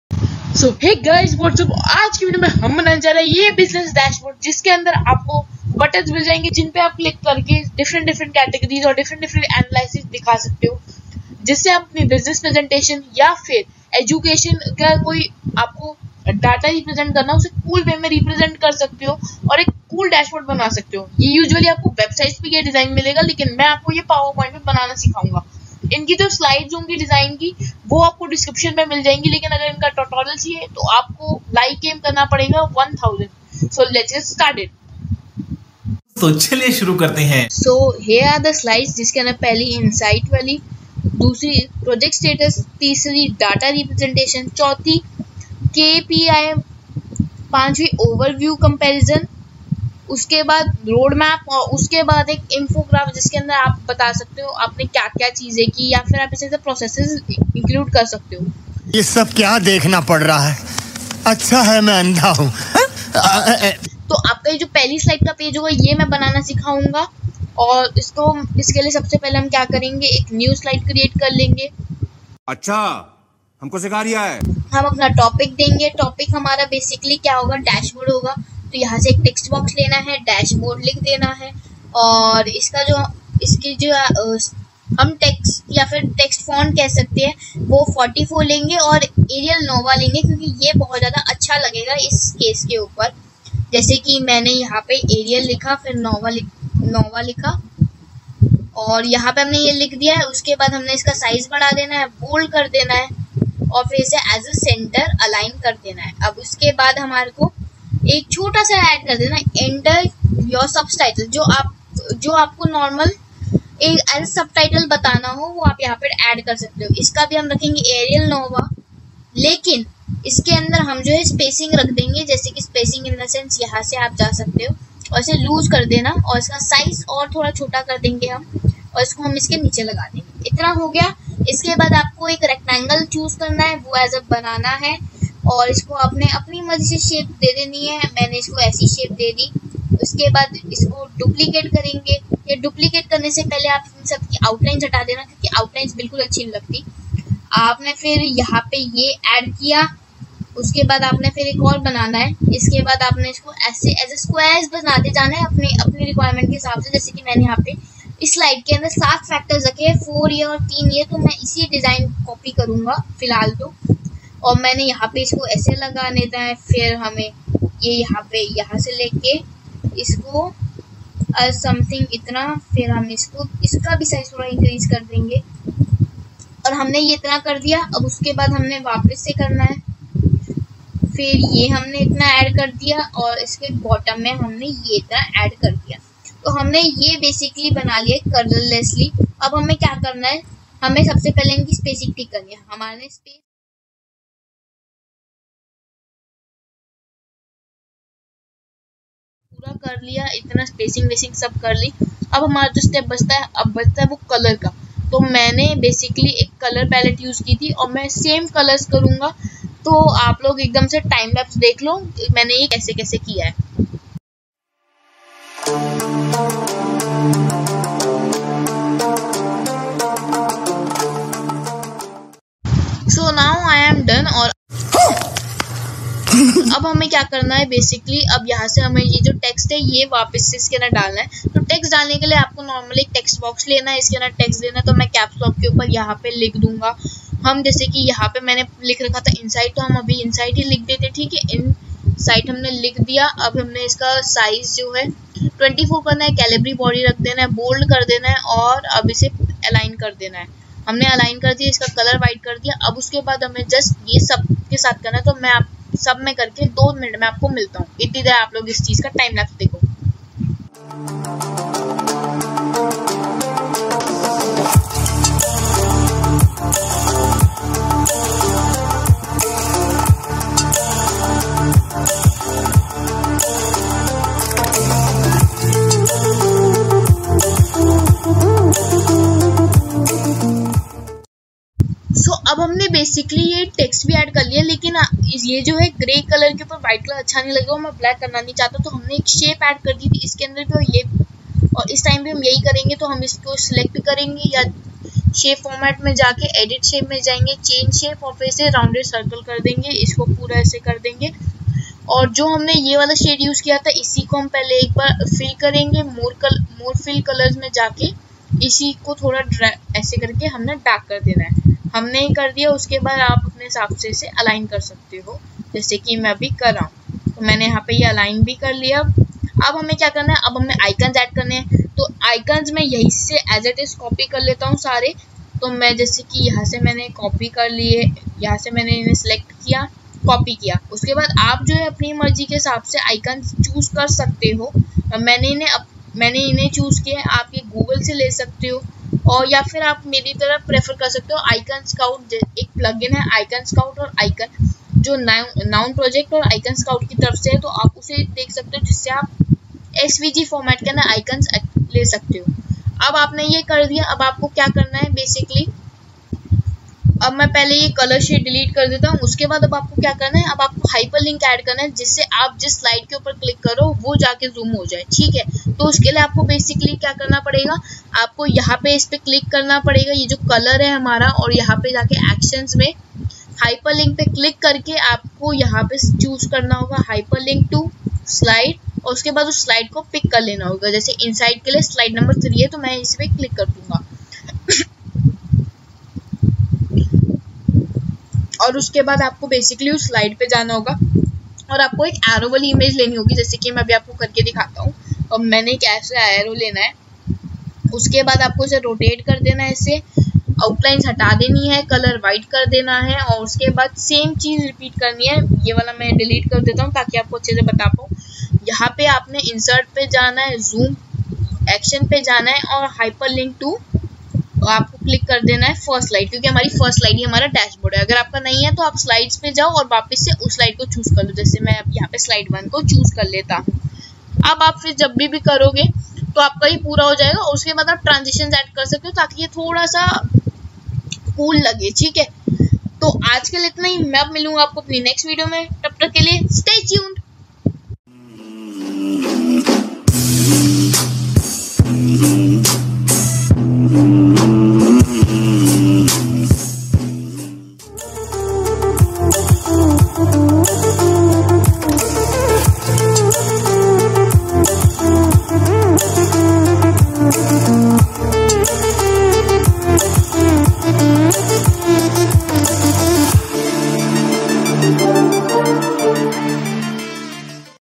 Interative Bussiness Dashboard | Powerpoint Infographic Dashboard | Free Slides
Download link provided below :
Hey Guys, What's Up and in today's video I would give a tutorial on how to create this awesome Interative Business Infographic Dashobard in PowerPoint. First we will start with some awesome infographic slides taken from some YT Channels and then build our dashboard slide. After that we will add hyperlink and customize them. So, Let's Get Started! By the way, in this tutorial I am on Windows and using PowerPoint Microsoft 365.